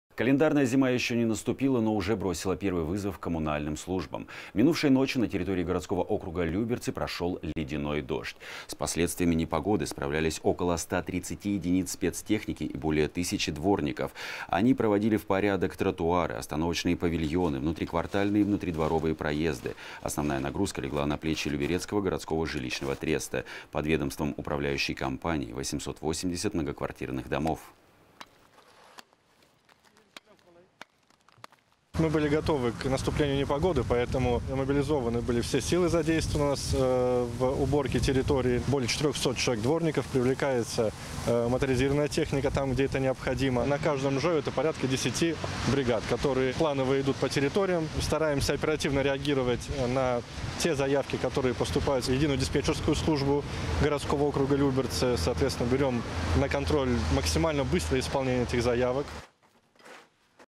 Первый вызов зимы
Календарная зима еще не настала, но уже бросила первый вызов коммунальным службам. Минувшей ночью на территории городского округа Люберцы прошел ледяной дождь. С последствиями непогоды справлялись около 130 единиц спецтехники и более тысячи дворников. Они приводили в порядок тротуары, остановочные павильоны, внутриквартальные и внутридворовые проезды. Основная нагрузка легла на плечи Люберецкого городского жилищного треста. Под ведомством управляющей компании – 880 многоквартирных домов.

Евгений Веснин, заместитель генерального директора АО «ЛГЖТ»:
"Мы были готовы к наступлению непогоды, поэтому мобилизованы все силы, задействованы в уборке территории.  Более 400 человек дворников, привлекается модернизированная техника там, где это необходимо. На каждом ЖЭУ порядка 10 бригад, которые планово идут по территориям. Стараемся оперативно реагировать на те заявки, которые поступают в Единую диспетчерскую службу городского округа Люберцы. Берем на контроль максимально быстрое исполнение этих заявок".